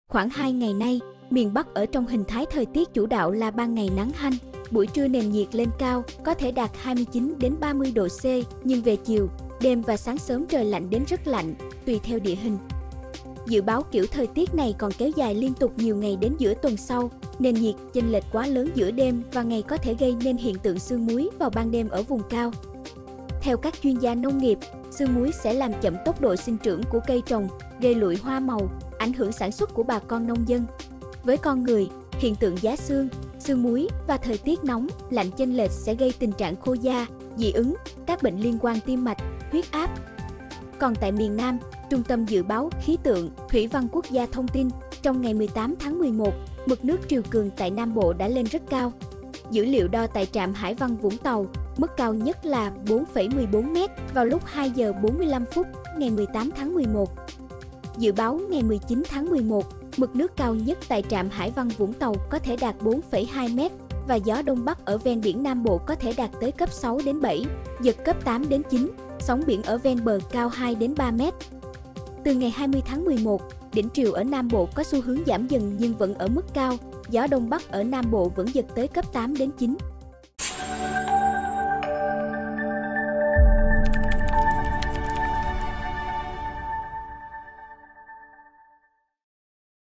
Miền Bắc có nguy cơ sương muối, miền Nam gió Đông Bắc giật cấp 8-9
Khoảng 2 ngày nay, miền Bắc ở trong hình thái thời tiết chủ đạo là ban ngày nắng hanh, buổi trưa nền nhiệt lên cao (có thể đạt 29-30°C) nhưng về chiều, đêm và sáng sớm trời lạnh đến rất lạnh, tùy theo địa hình.